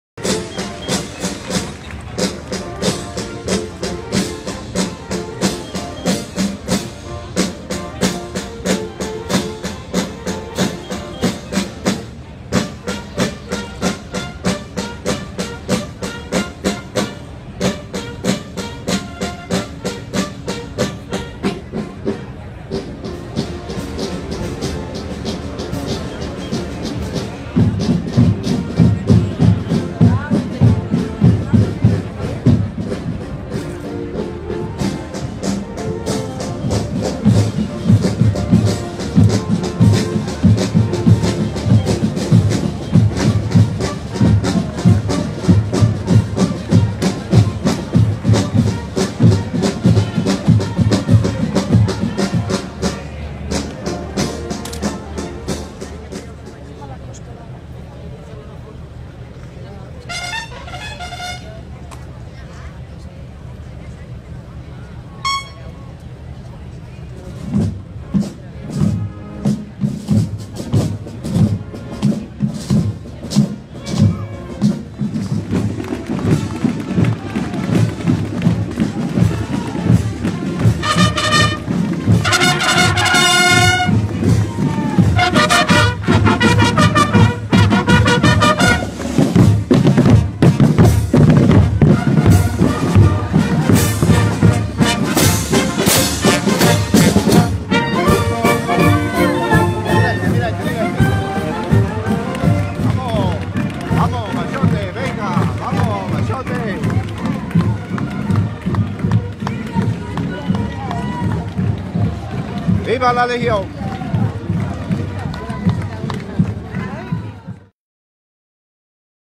Desembarco de la Legión
Jueves Santo de la Semana Santa de Málaga de 2017